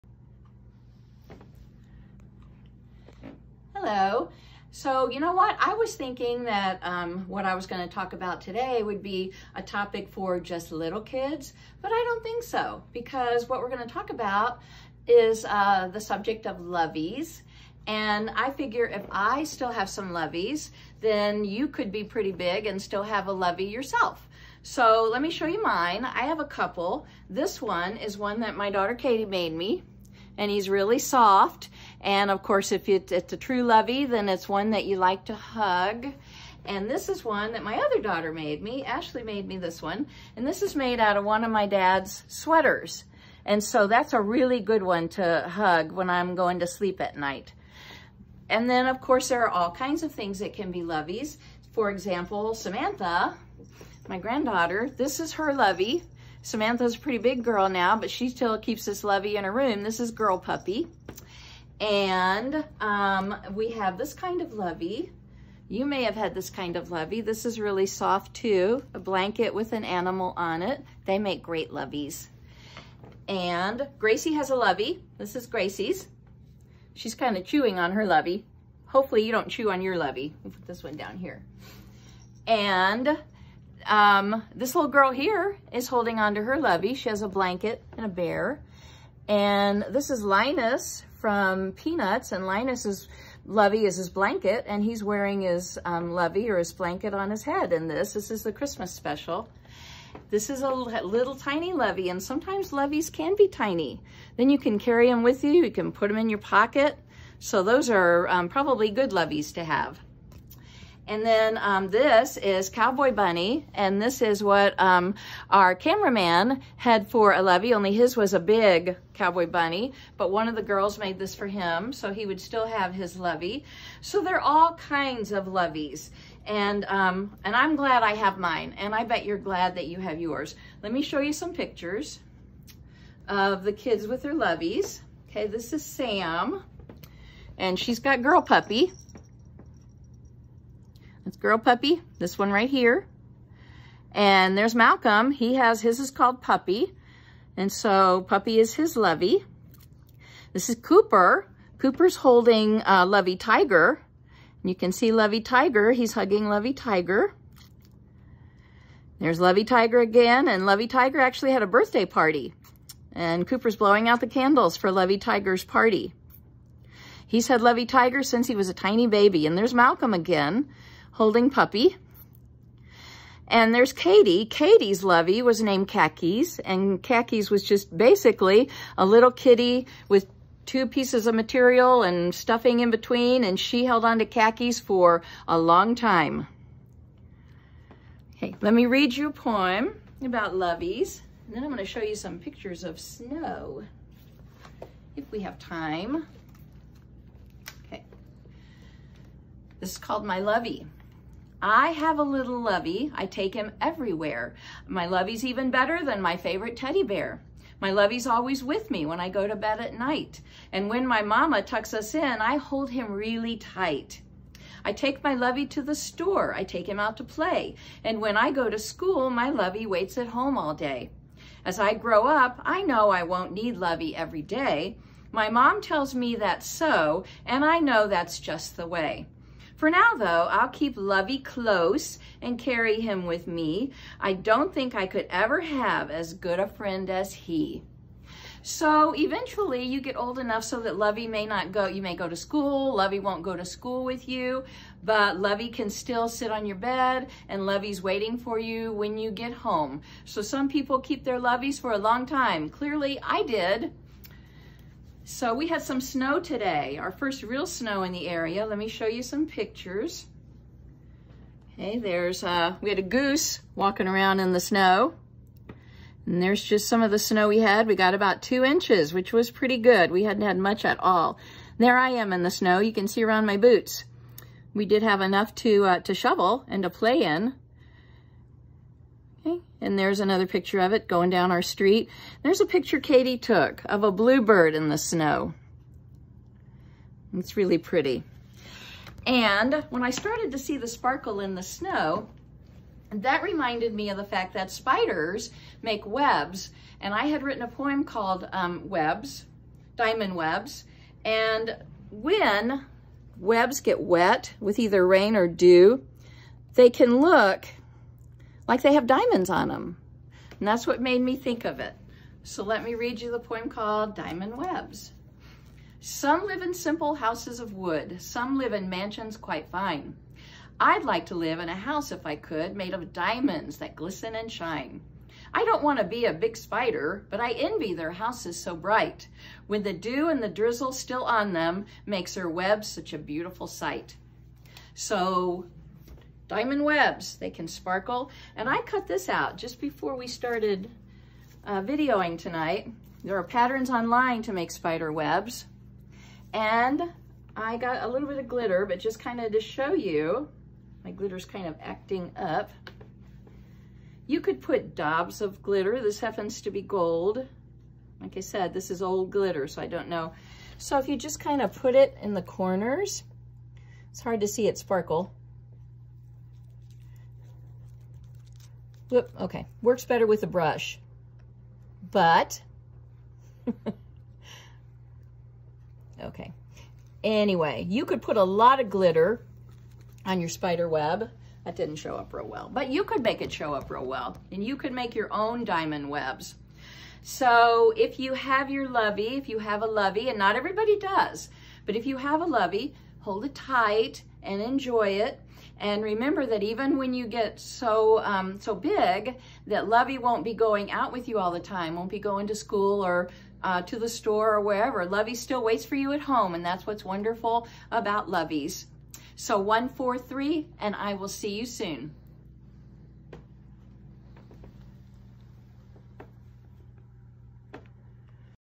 GG Sprinkles - My Lovey & Diamond Webs
GG reads a poem about loveys and shows different kinds of loveys. She talks about how we may not—and don’t have to—outgrow them but we give them a different place in our lives.